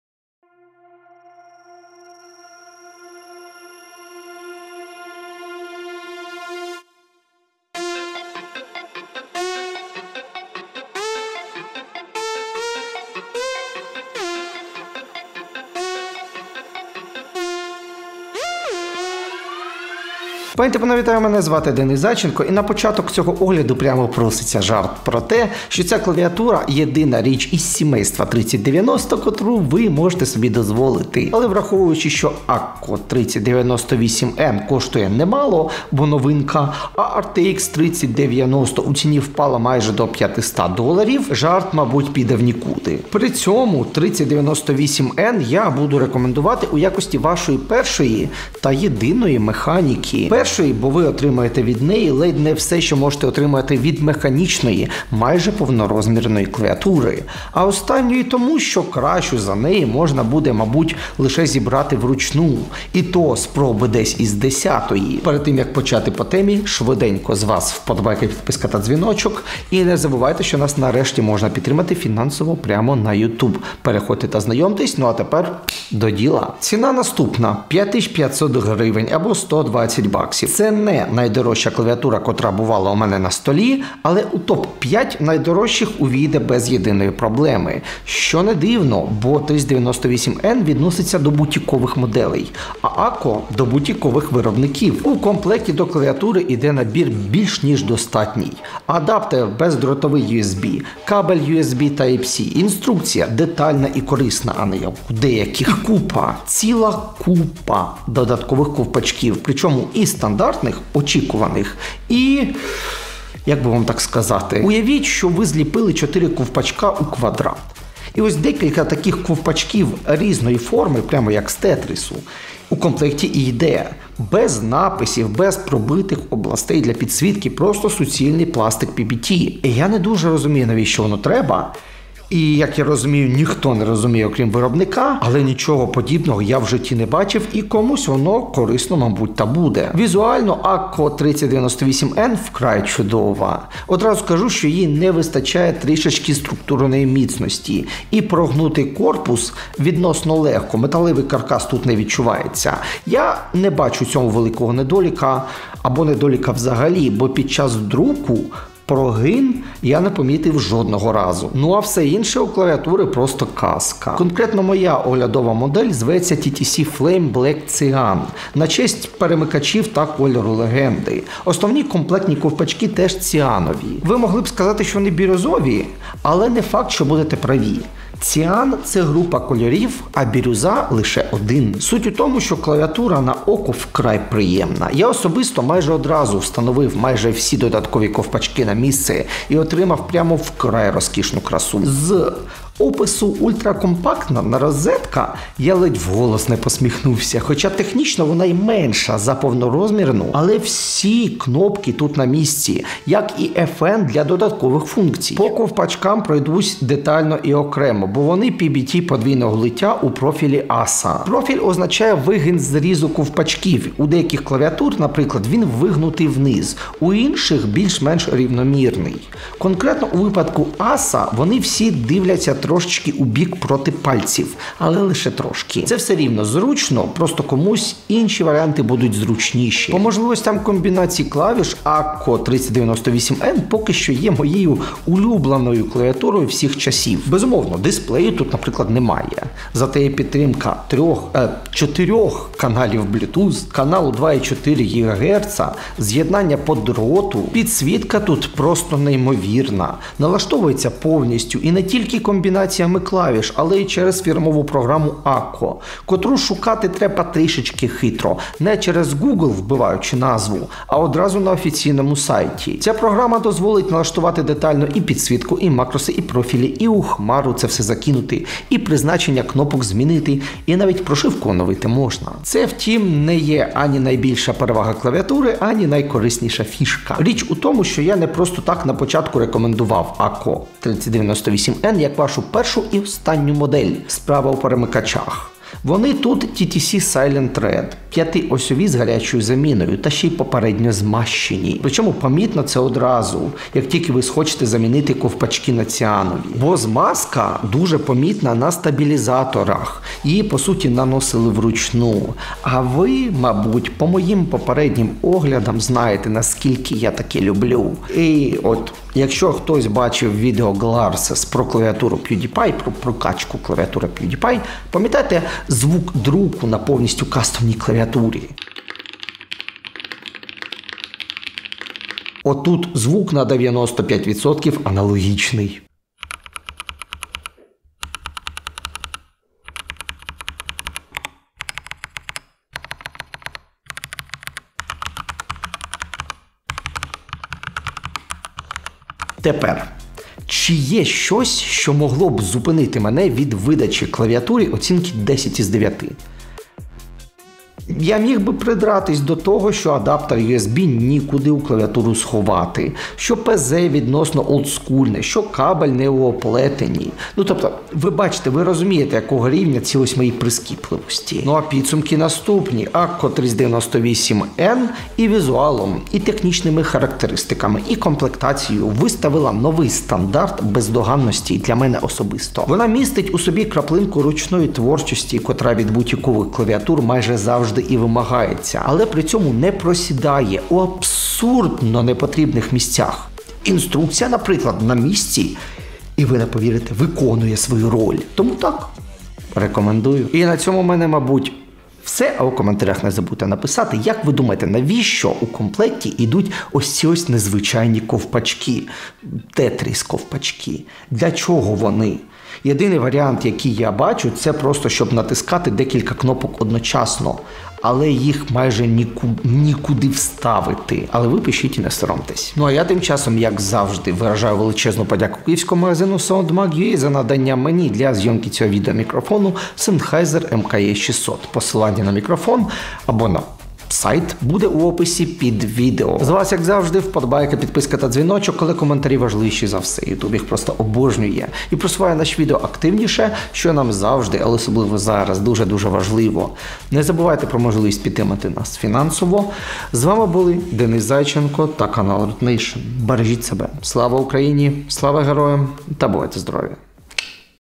⌨️ Огляд механічної клавіатури Akko 3098N Black & Cyan
00:00 Вступ
01:15 Ціна і комплектація
02:19 Зовнішність та характеристики
04:07 Підсвітка та ПЗ
04:37 Перемикачі
05:39 Недоліки
06:01 Підсумки